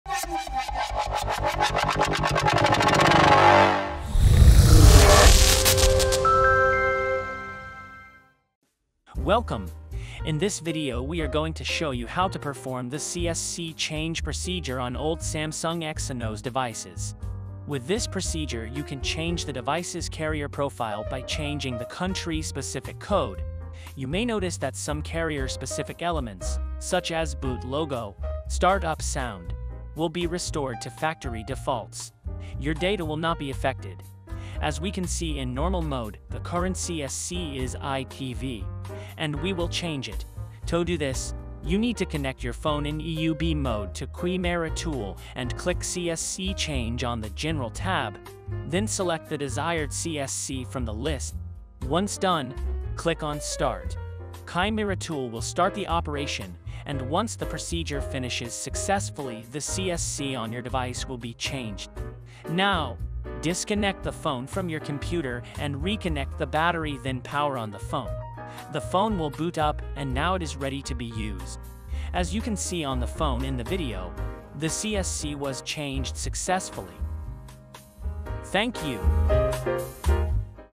Samsung CSC change on OLD EXYNOS devices with ChimeraTool [NO ROOT, ANY KNOX, ANY BIT, ANY VERSION]
The following devices are supported:

Samsung Galaxy A50 (SM-A505F, SM-A505FM, SM-A505FN, SM-A505G, SM-A505GN, SM-A505GT, SM-A505N, SM-A505U, SM-A505U1, SM-A505W, SM-A505YN, SM-S506DL)
Samsung Galaxy A50s (SM-A507FN, SM-A5070)
Samsung Galaxy A51 2019 (SM-A515F, SM-A515U, SM-A515U1, SM-A515W, SM-S515DL)
Samsung Galaxy F41 (SM-F415F)
Samsung Galaxy XCover Pro (SM-G715FN, SM-G715U, SM-G715U1, SM-G715W)
Samsung Galaxy M21 (SM-M215F, SM-M215G)
Samsung Galaxy M30s (SM-M307F, SM-M307FN, SM-M3070)
Samsung Galaxy M31 (SM-M315F)
Samsung Galaxy Tab S6 Lite 10.4 (SM-P610, SM-P615, SM-P615C, SM-P615N, SM-P617)

Available procedures: Restore / Store Backup, Unlock / Relock Bootloader, CSC Change, Change Serial, Factory Reset, Remove FMM, Remove FRP, Knoxguard Remove, Remove Lost Mode, Remove Common Criteria, Remove MDM, Remove Warnings, Restore Security Backup, Read Codes, Network Factory Reset (**)
 *: In most cases, it's necessary to disassemble the phone and use hardware-testpoints to put the phone into EUB mode. Visit our guides here and our Test Point Database to learn more.
**: if Read Codes is not available on your device, please try Network Factory Reset (if the procedure button is visible) to achieve the same functionality. There are devices where none is available.